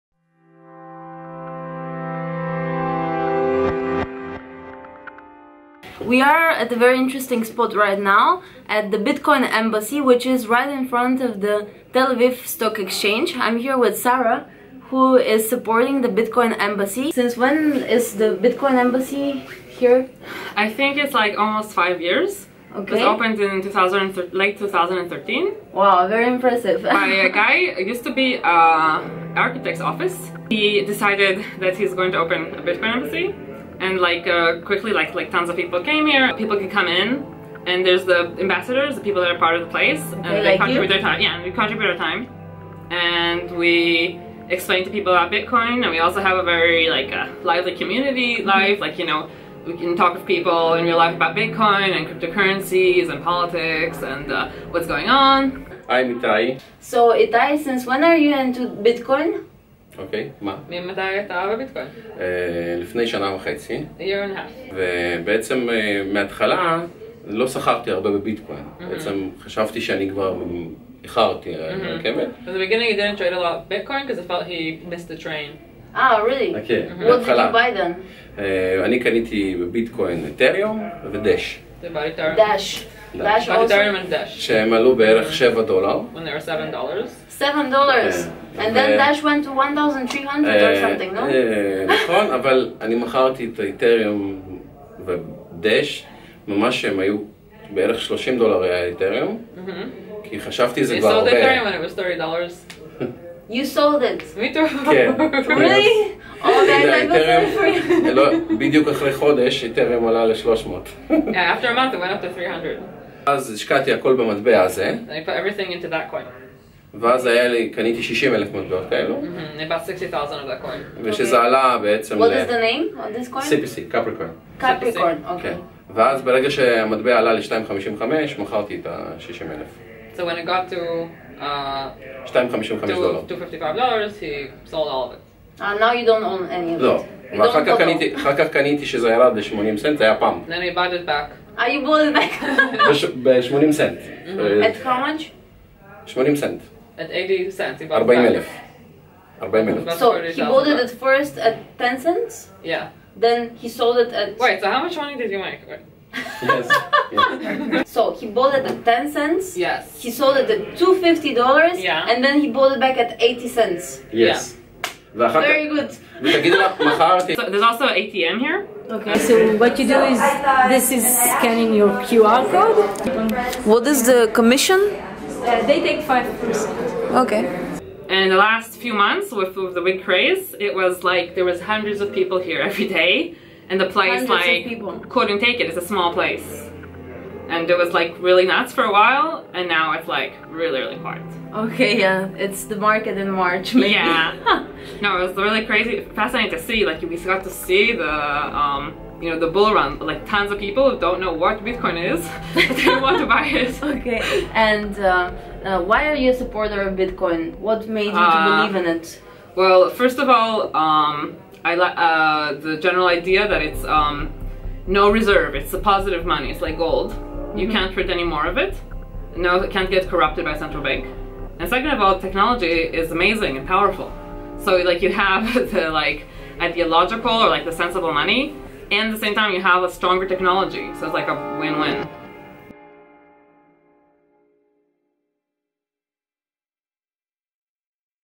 Bitcoin Embassy in Israel - A Bitcoin Nation
**Turn on the SUBs**      Meet the early adopters of Bitcoin - the Bitcoin Embassy in Tel Aviv, which is situated right outside the Tel Aviv Stock Exchange.

Meet a Bitcoin Ambassador - Sarah and a cryptocurrency bullish trader - Itay, who tells his personal story: Guess how many times he multiplied his money for an year and a half? Stories from the Bull run. See one of the first Bitcoin ATMs in the world.

The interviews were recorded in early April 2018.